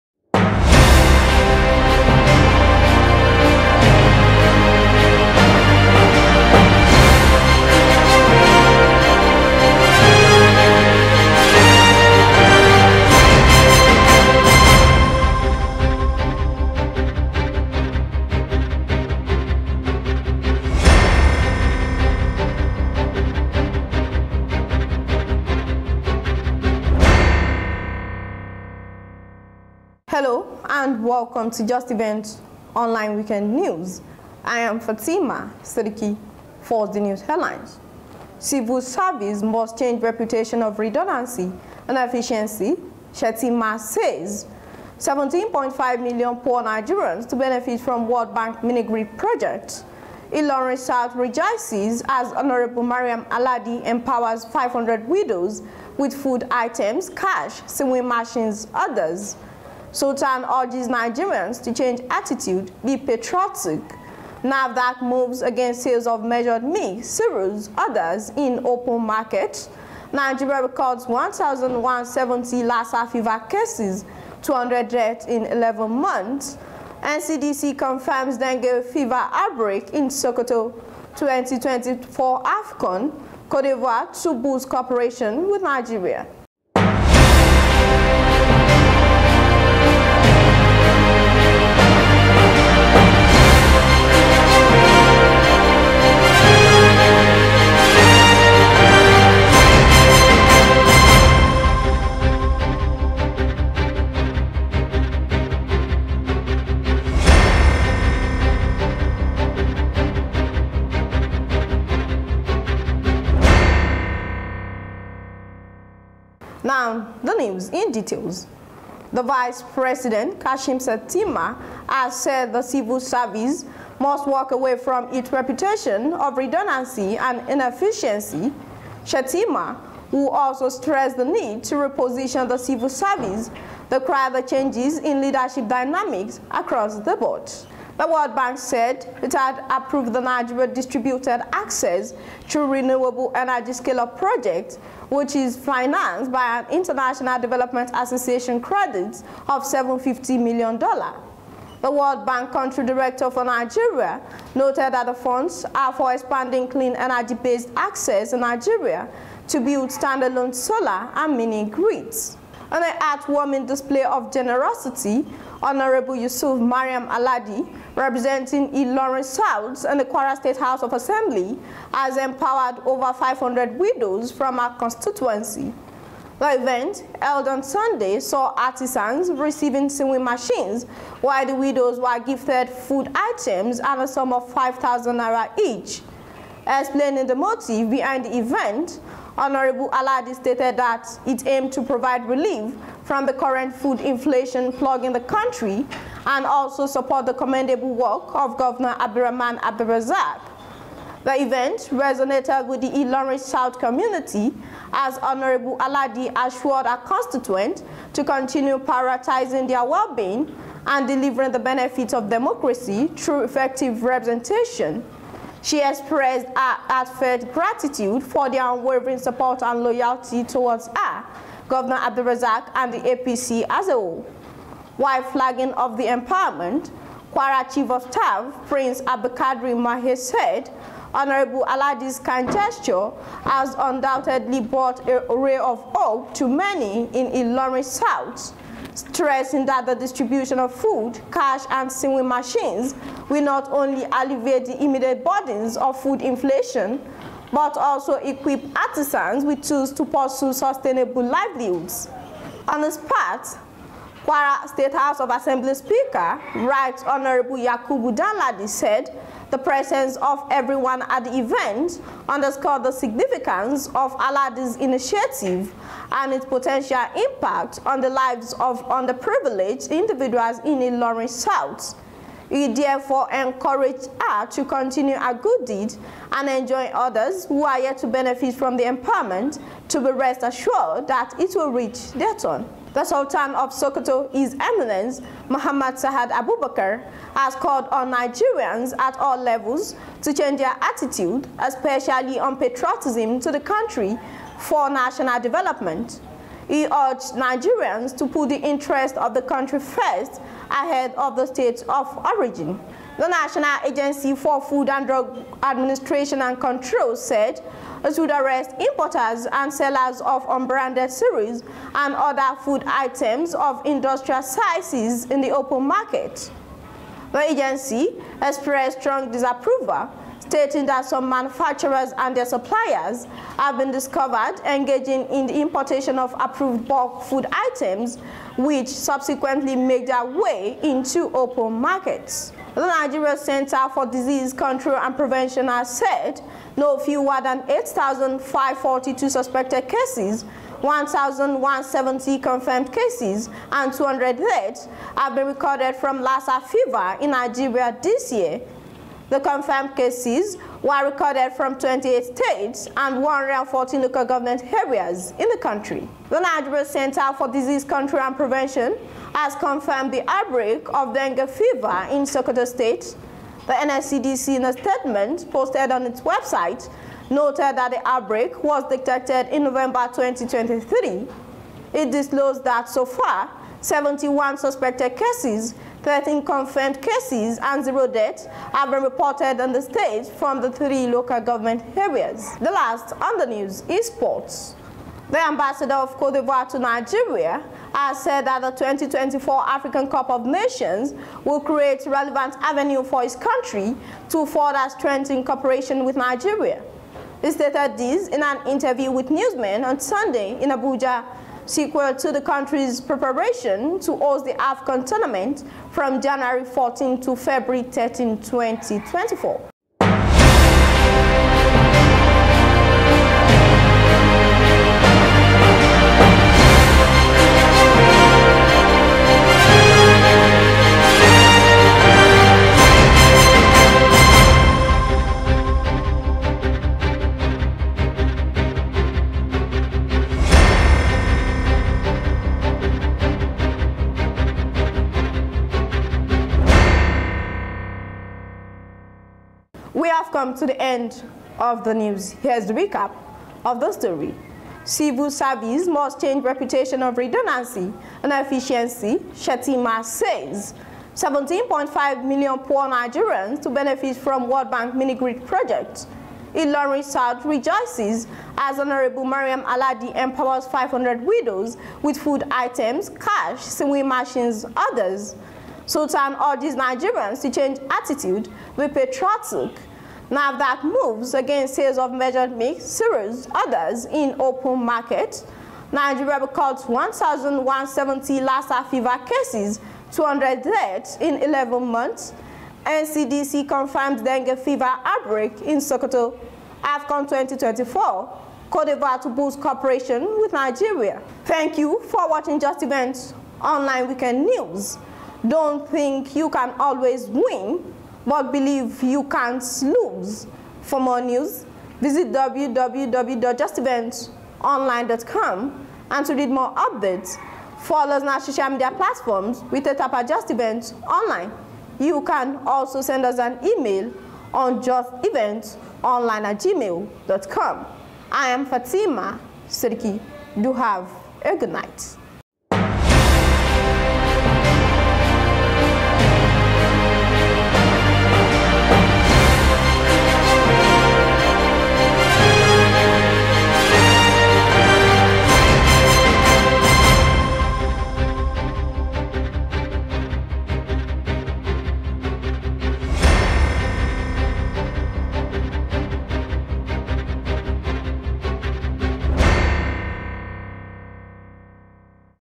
JEO TV Weekend News
Justeventonline weekend News
17/12/2023

News *•Civill service must change reputation of redundancy, inefficiency — Shettima*

News *•17.5 m poor Nigerians to benefit from World Bank mini-grid project*

News *•Ilorin South Rejoices as Hon. Maryam Aladi Empowers 500 Widows with Food Items, Cash, Sewing Machines, others*

News *•Sultan urges Nigerians to change attitude, be patriotic*

News *•NAFDAC moves against sales of measured milk, cereals, others in open markets*

Health *•Nigeria records 1,170 Lassa fever cases, 200 deaths in 11 months*

Health *•NCDC confirms dengue fever outbreak in Sokoto*

Sport *•AFCON 2024: Côte d’Ivoire to boost cooperation with Nigeria*

Word of the day: *Don't think you can always win, but believe you can't lose*